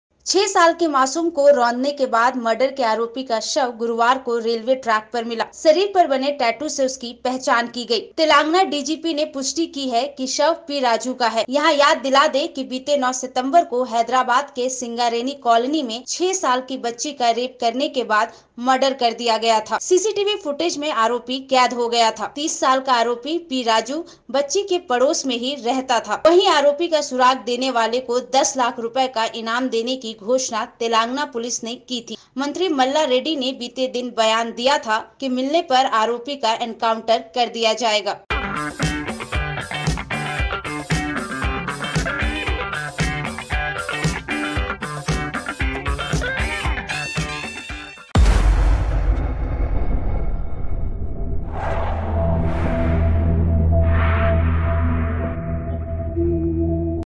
6 साल की बच्ची का किया था रेप और मर्डर, आज इस हाल में मिला... देखें वीडियो
6 साल की बच्ची का किया था रेप और मर्डर, आज इस हाल में मिला... देखें वीडियो